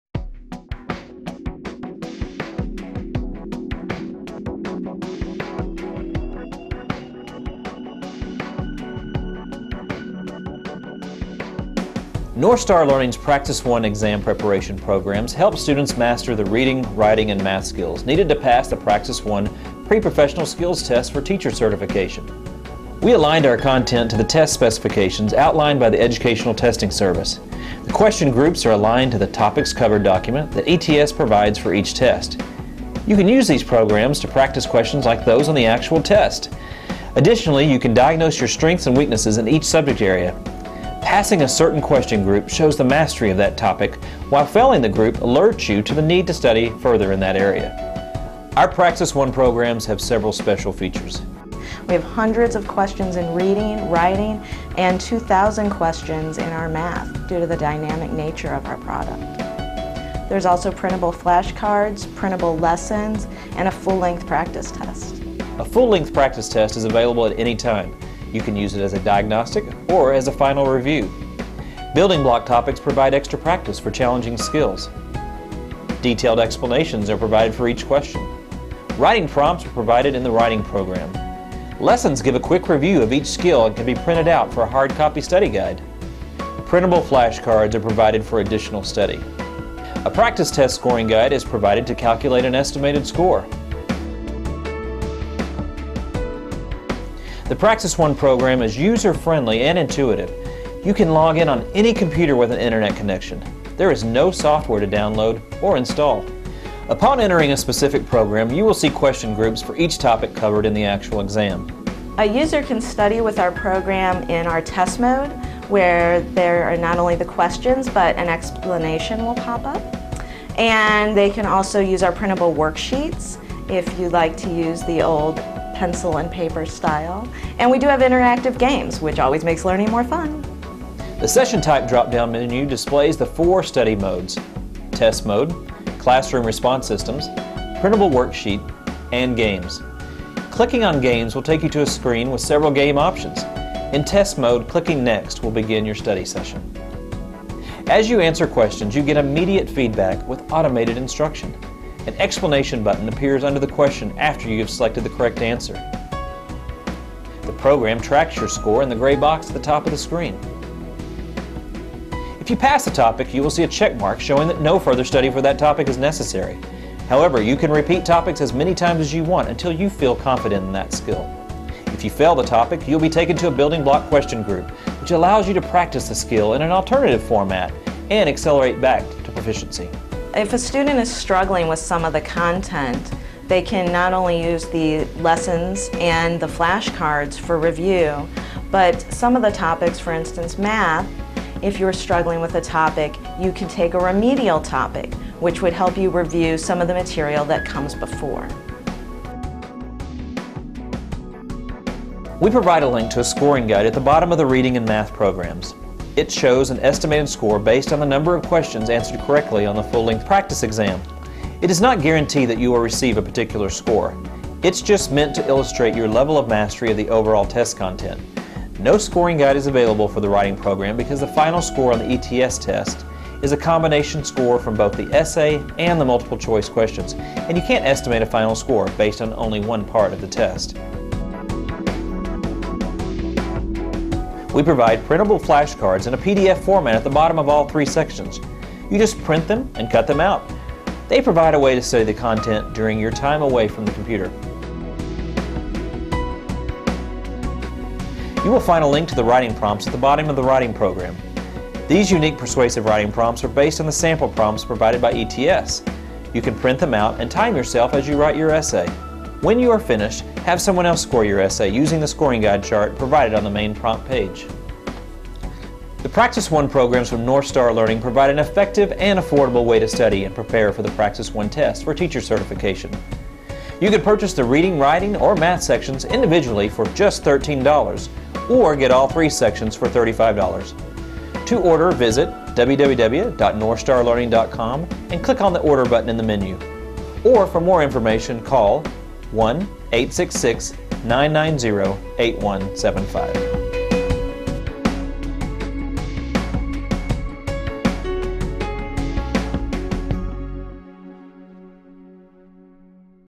Northstar Praxis I Preparation
The Northstar Learning Praxis I program is designed to help teacher candidates master the fundamental skills tested on the Pre-Professional Skills Test in reading, writing, and mathematics.